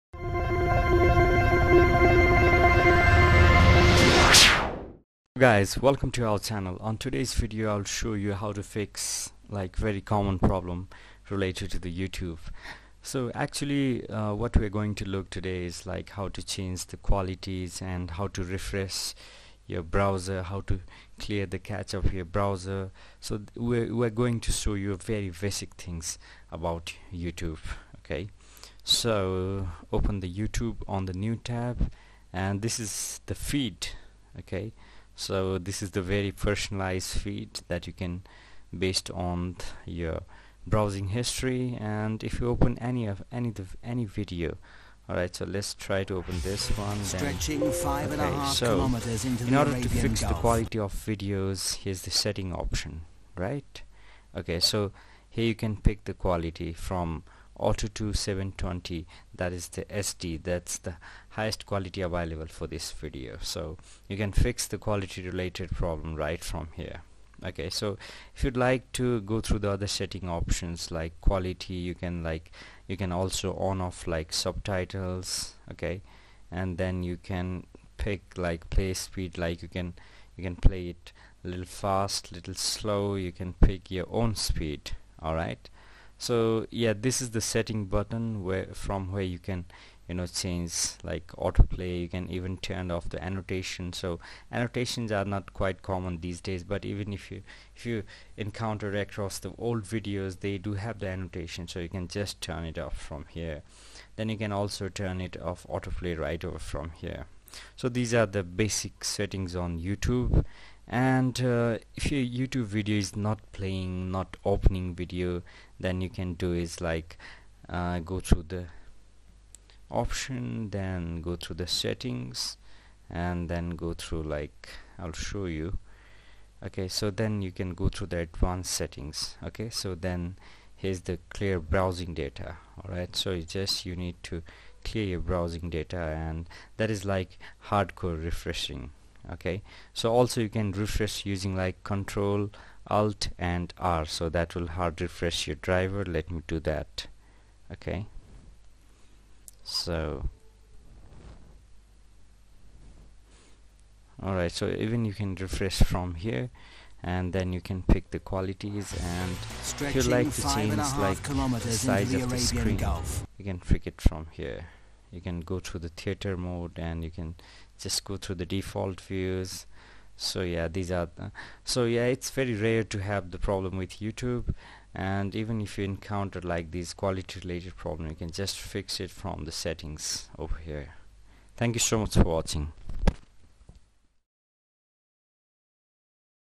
Fixing Youtube Buffering Issue 2019 (How To)
Sometimes it might take a lot of time to load a video while using youtube. Due to some necessary or cached data that are stored on your computer makes any websites or videos slow. For that follow the instructions below:

Step 1: Open 'Google Chrome'
Step 2: Then click on the three dots situated at the corner of the screen and select 'Settings'
Step 3: Scroll down and click on 'Advanced'.
Step 4: Then find 'Clear Browsing Data' and click to it.
Step 5: Mark upon the browsing history, cookies and the other sites, cached images and files and select 'Clear Data'.
                                              Or
Directly click on the Ctrl+Alt+R. This refreshes the page that you are using at the current moment.